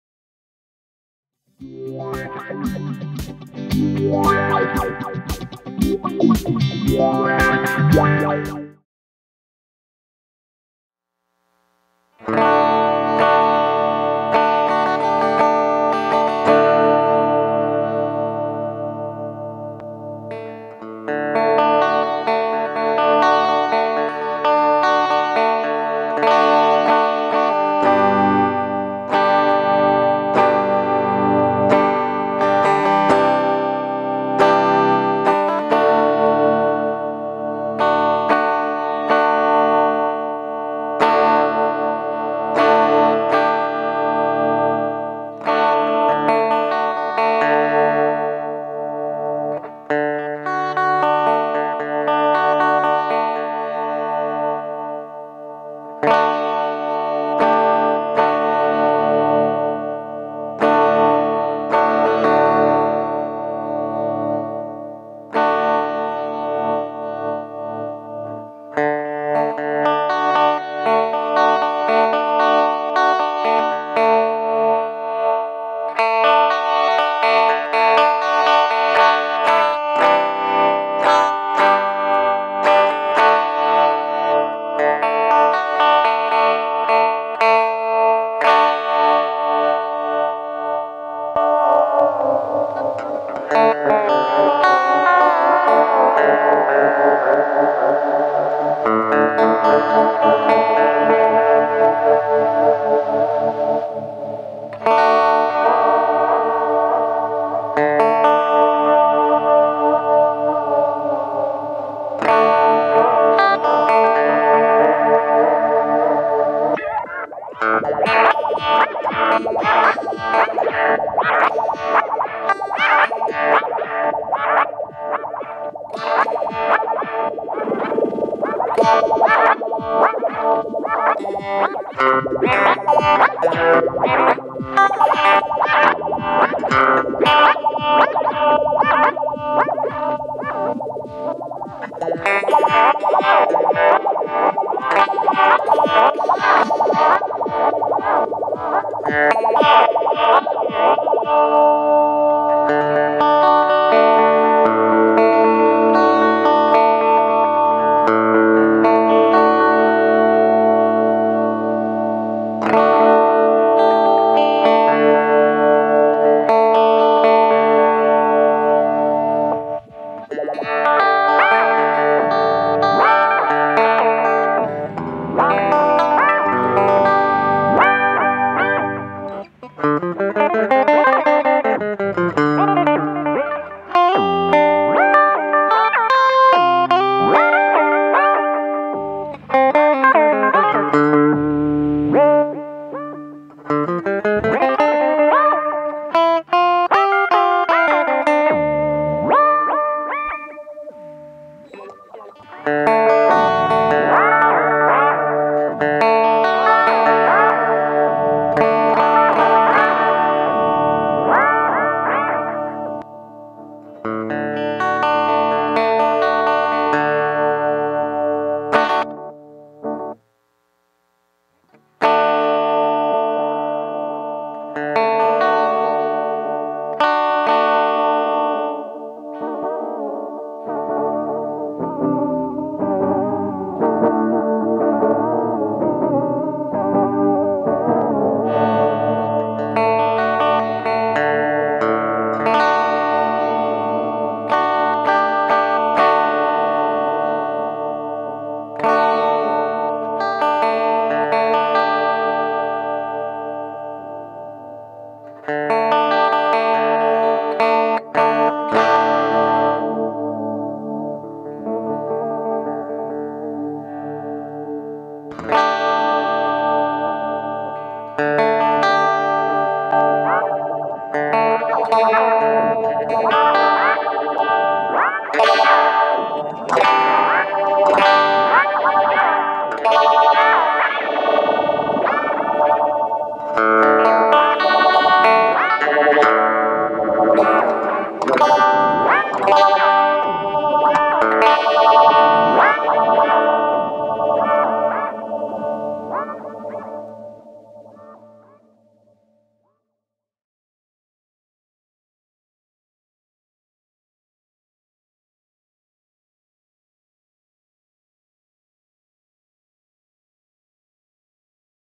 POLY Expressive MIDI Foot Controller
This is a demo video of the MIDI Foot Controller from POLY Expressive performed and produced by Pedal of the Day. for more effects pedal reviews and demos, and be sure to check out more of the videos here on our YouTube channel before you leave. Cheers!

Recorded with:

PHRED Instruments Ernesto VH3 Koa Brown Burst loaded with Seymour Duncan ’59 SH-1n and SH-1b Humbucker pickups

Roland JC-40 Jazz Chorus

Chase Bliss Audio Thermae / Wombtone / Gravitas / Tonal Recall